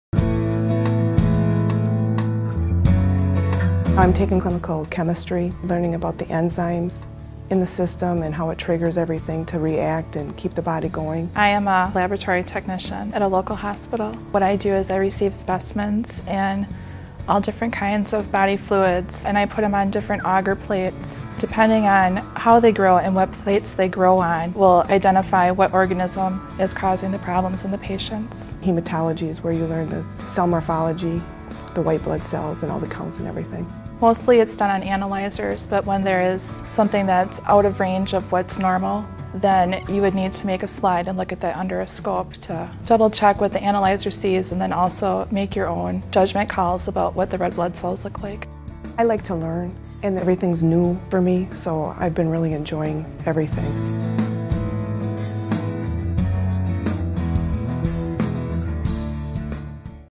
Medical Lab Technician
This is a collaborative program offered through the Health Occupations Department of Health, Human and Protective Services division at MATC Madison.

Graduates of the program qualify for both the American Society of Clinical Pathologists Board of Registry and the National Certification Agency for Laboratory Personnel certification exams for medical laboratory technicians and clinical laboratory technicians, respectively, under the direction of the American Society for Clinical Laboratory Science.